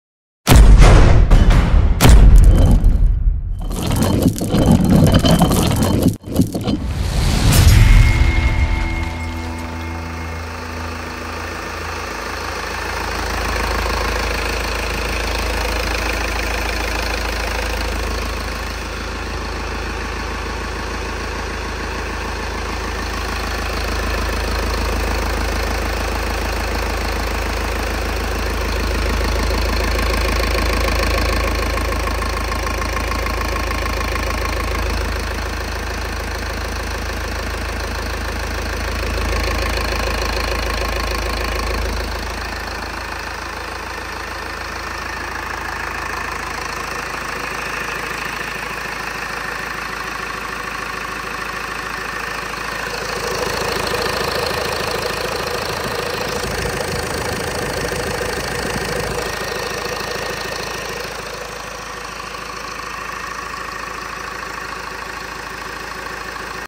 (P-60015) 2006 MERCEDES MBR 900 ENGINE ASSEMBLY
(Used) 2006 Mercedes MBE 900, ENGINE ASSEMBLY- Displacement: 4.3L,  Horsepower: 170, ESN: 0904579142, Reference images for condition  For more information about this item, give us a call. We ship Worldwide! (CRG HEAVY TRUCK PARTS) 929 W PULASKI HWY  ELKTON, MD 21921